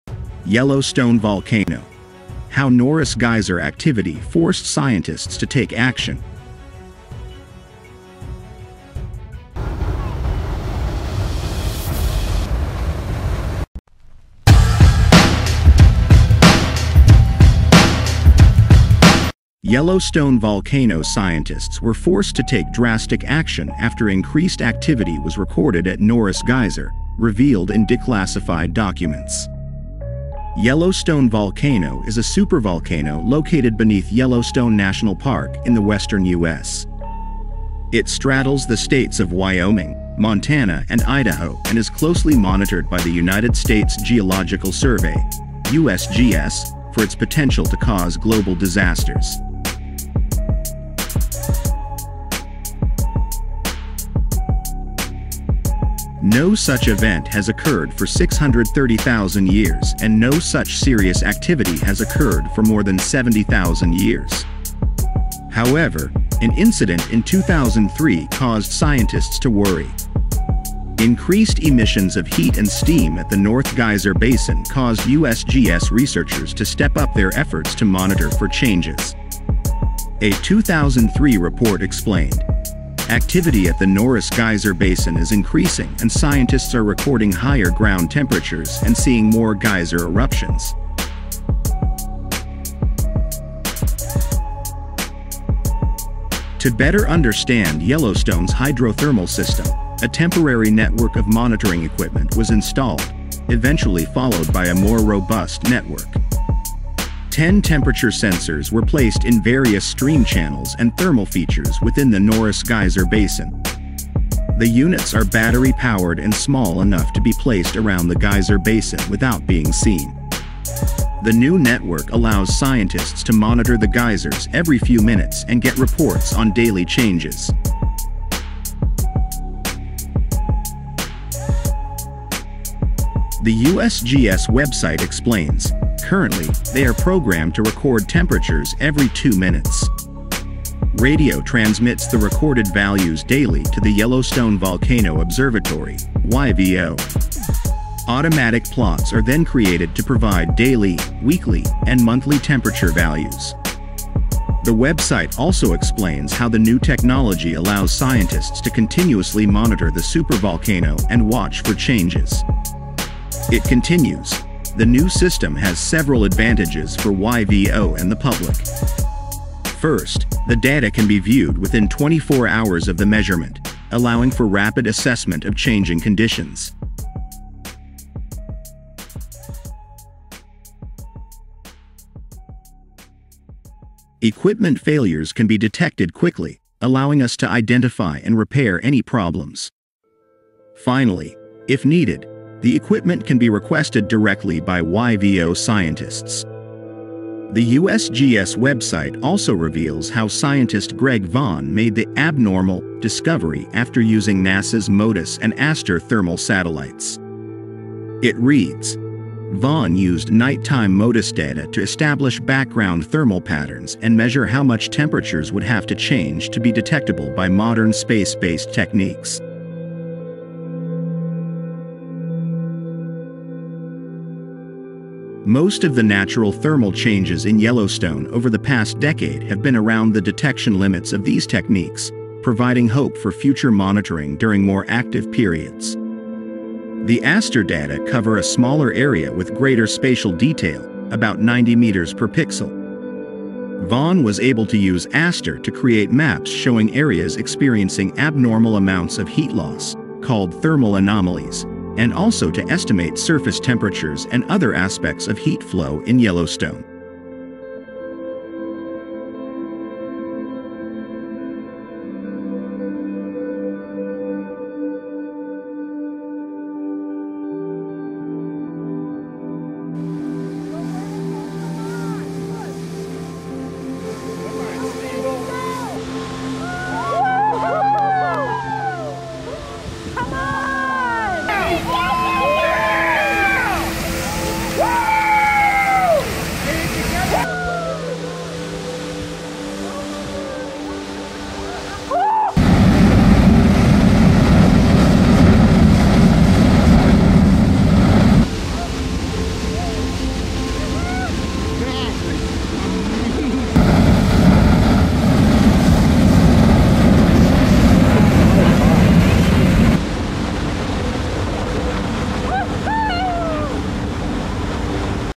Horrible: Live footage Geyser Basin's 3nd erupt,threatening lives Yellowstone National Park visitors
YELLOWSTONE volcano scientists were forced to take drastic action after increased activity was recorded at Norris Geyser, revealed in declassified documents. Yellowstone volcano is a supervolcano located beneath Yellowstone National Park in the western US. It is located between the states of Wyoming, Montana and Idaho and is closely monitored by the United States Geological Survey (USGS) for its potential to cause disaster on a global scale.

The last such event had not occurred for 630,000 years and there had been no serious activity for more than 70,000 years. However, an incident in 2003 caused scientists to worry. Increased emissions of heat and steam at the North Geyser Basin caused USGS researchers to step up their efforts to monitor for any changes. A 2003 report explains: Activity in the Norris Geyser Basin has increased, and scientists are recording higher ground temperatures and witnessing more geyser eruptions.